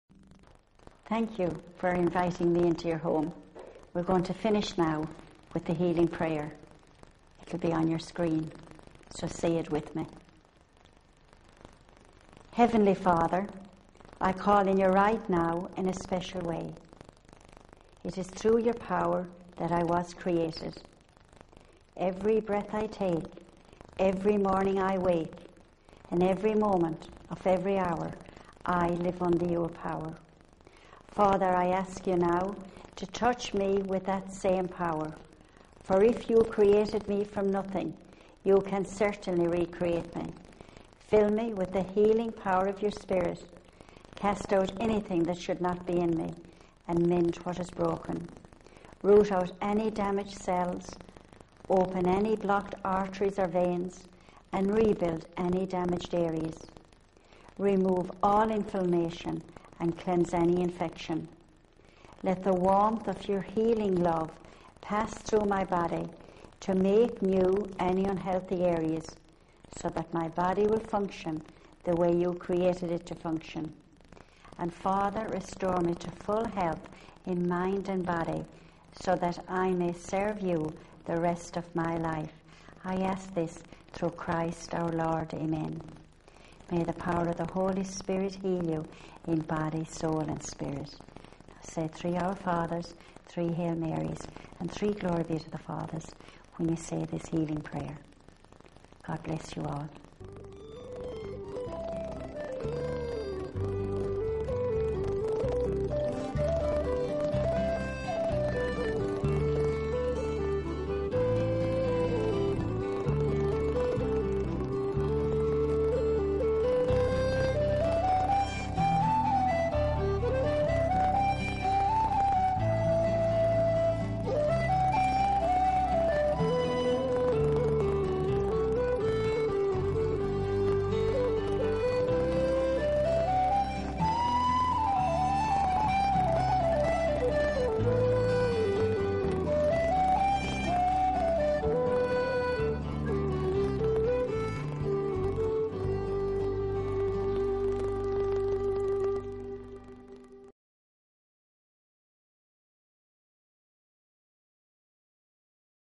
Healing Prayer
Join Mary Malone in prayer.